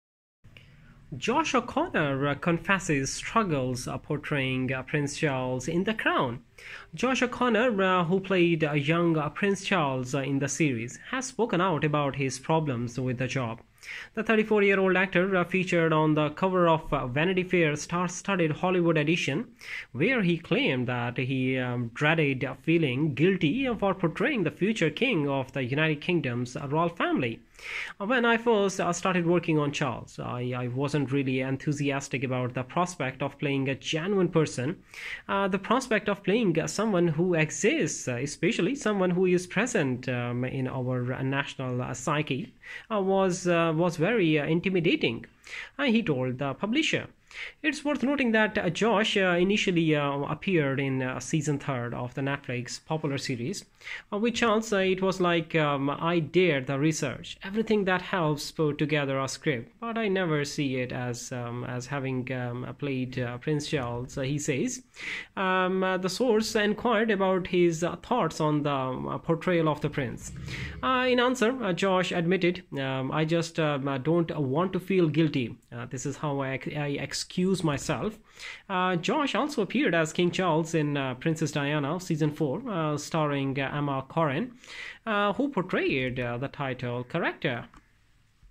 Josh O'Connor confesses struggles portraying Prince Charles in 'The Crown'
Josh O'Connor, who played young Prince Charles in the series, has spoken out about his problems with the job.
The 34-year-old actor featured on the cover of Vanity Fair's star-studded Hollywood edition, where he claimed that he dreaded feeling "guilty" for portraying the future King of the United Kingdom's Royal Family.
"When I first started working on Charles, I wasn't really enthusiastic about the prospect of playing a genuine person. "The prospect of playing someone who exists, especially someone who is present in our national psyche, was very intimidating," he told the publisher.
It's worth noting that Josh initially appeared in season three of the Netflix popular series.
"With Charles, it was like, I did the research, everything that helps put together a script, but I never see it as having played Prince Charles," he says.
The source enquired about his thoughts on the portrayal of the Prince.
In answer, Josh admitted, "I just don't want to feel guilty." "This is how I excuse myself."
Josh also appeared as King Charles in Princess Diana season four, starring Emma Corrin, who portrayed the title character.